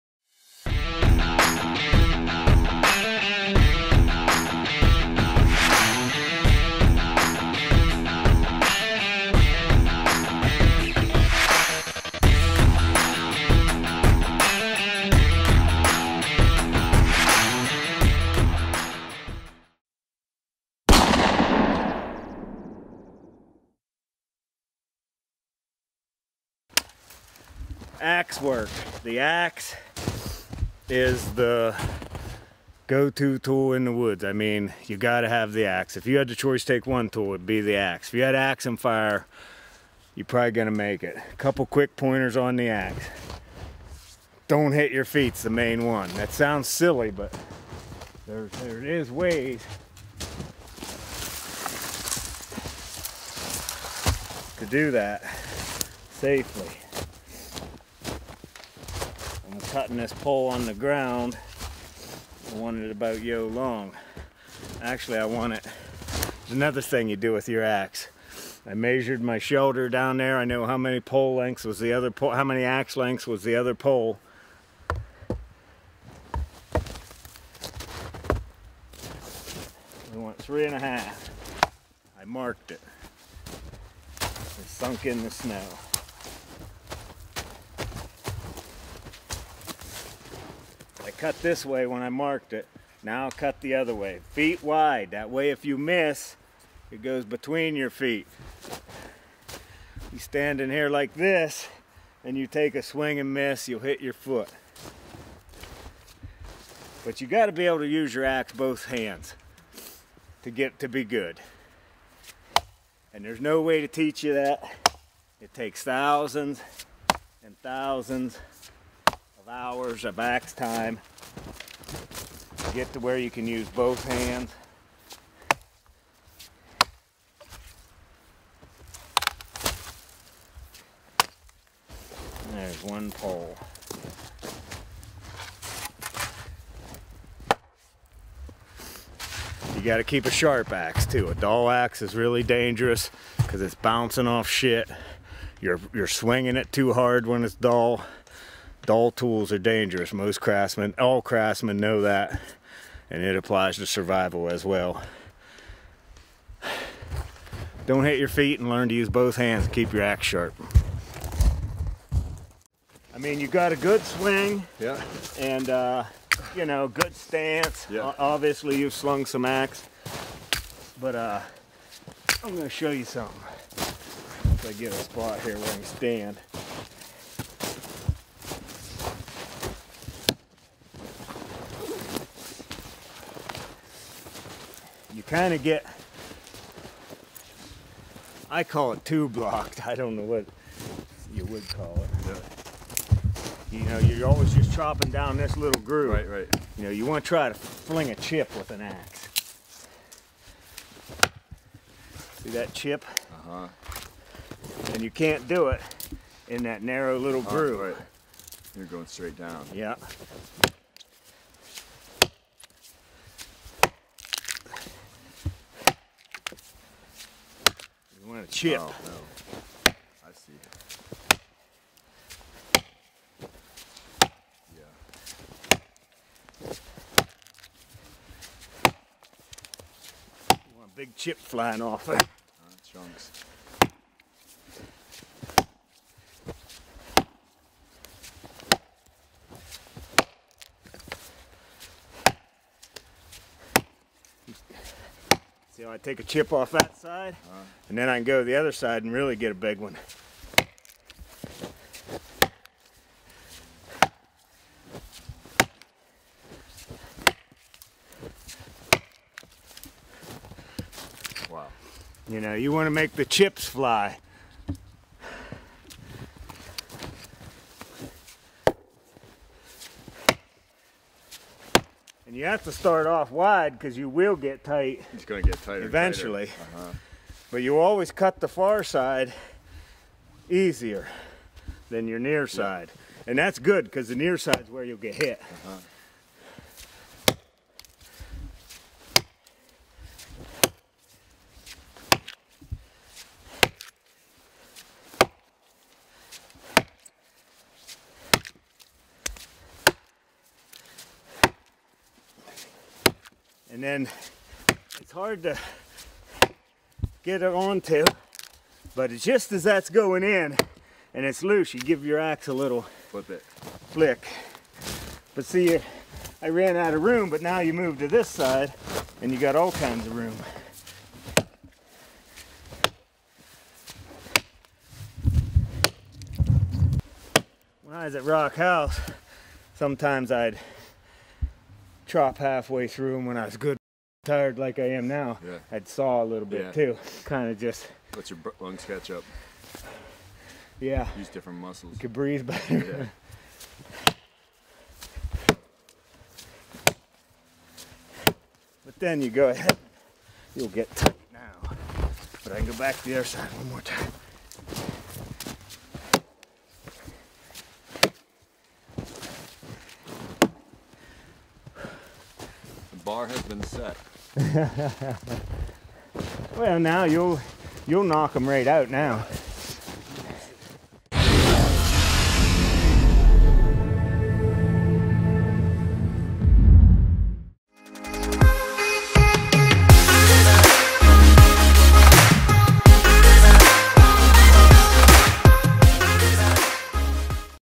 THE BULLET POINT: Roland 'Rock House' Welker Shows Us How to Use an Ax
Season 7 "Alone" series winner, outdoorsman, big game hunter, survivalist Roland Welker teaches us one of survival's most important skills: ax work.